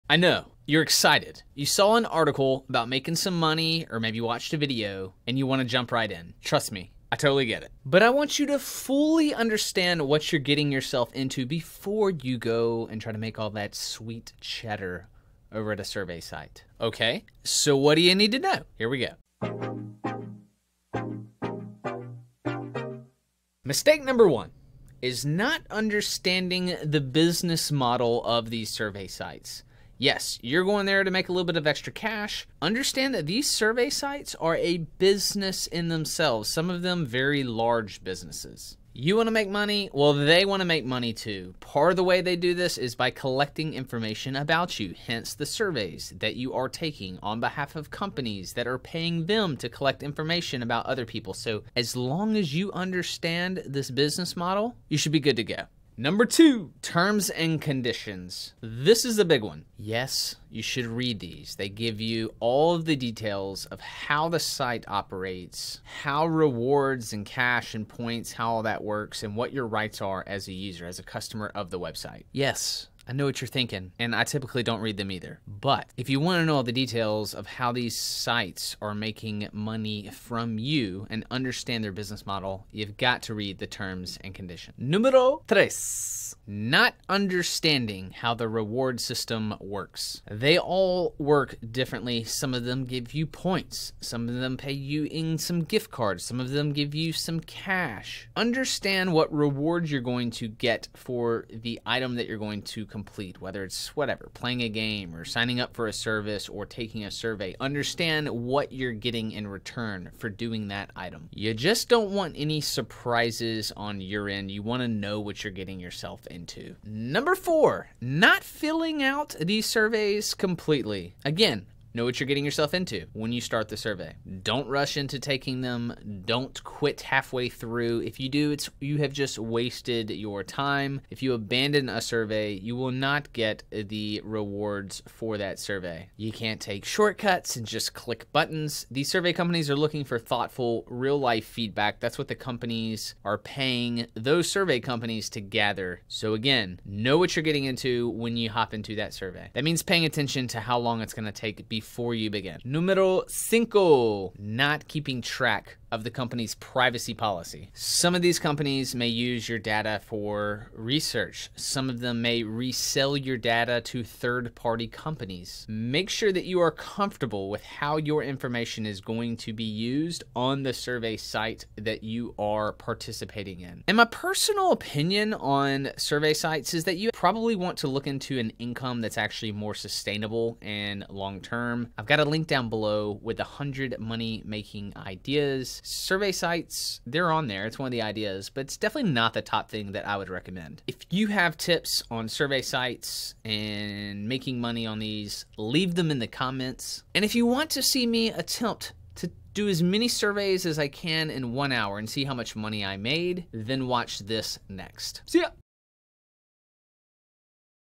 Avoid These Survey Site Mistakes
If you're going to try to make money with survey websites, be sure to avoid these mistakes.